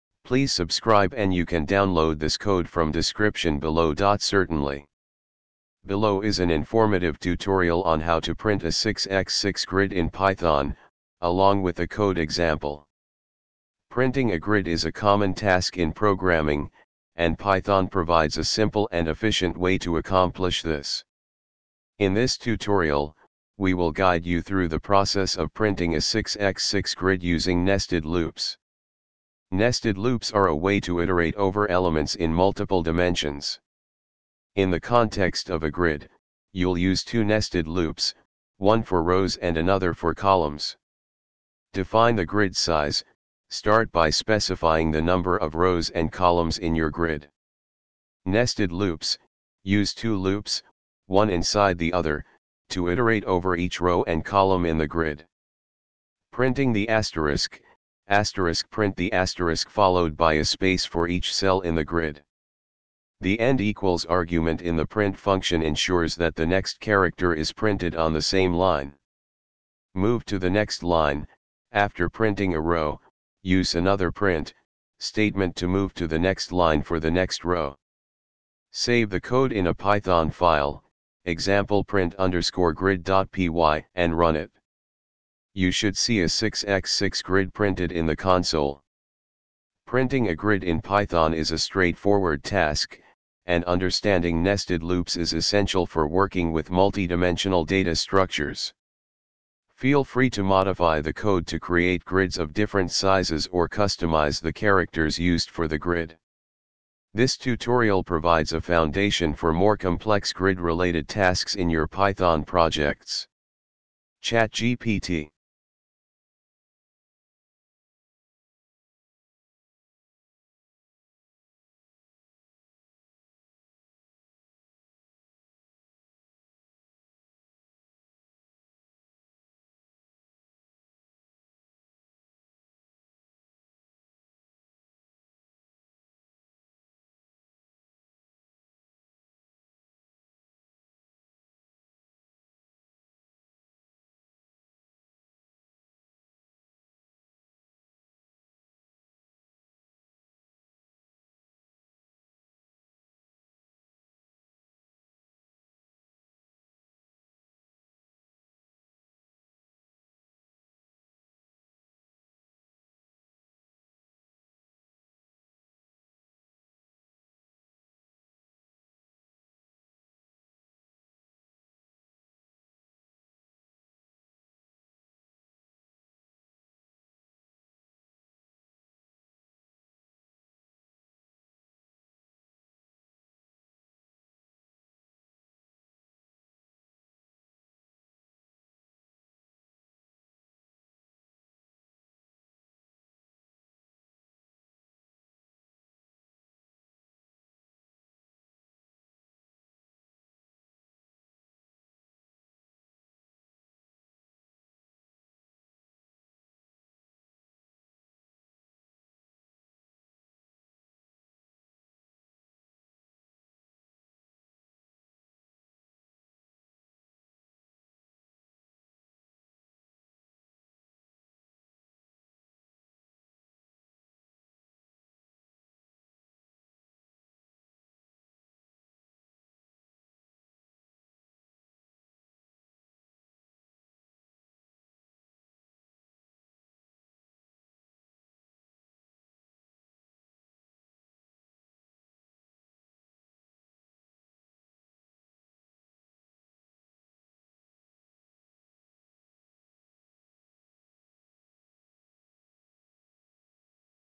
How to print a 6 6 grid as described below in Python
Certainly! Below is an informative tutorial on how to print a 6x6 grid in Python, along with a code example:
Printing a grid is a common task in programming, and Python provides a simple and efficient way to accomplish this. In this tutorial, we will guide you through the process of printing a 6x6 grid using nested loops.
Nested loops are a way to iterate over elements in multiple dimensions. In the context of a grid, you'll use two nested loops – one for rows and another for columns.
Define the grid size: Start by specifying the number of rows and columns in your grid.
Nested loops: Use two loops, one inside the other, to iterate over each row and column in the grid.
Printing the asterisk (*): Print the asterisk followed by a space for each cell in the grid. The end='' argument in the print function ensures that the next character is printed on the same line.
Move to the next line: After printing a row, use another print() statement to move to the next line for the next row.
Save the code in a Python file (e.g., print_grid.py) and run it. You should see a 6x6 grid printed in the console.
Printing a grid in Python is a straightforward task, and understanding nested loops is essential for working with multi-dimensional data structures. Feel free to modify the code to create grids of different sizes or customize the characters used for the grid. This tutorial provides a foundation for more complex grid-related tasks in your Python projects.
ChatGPT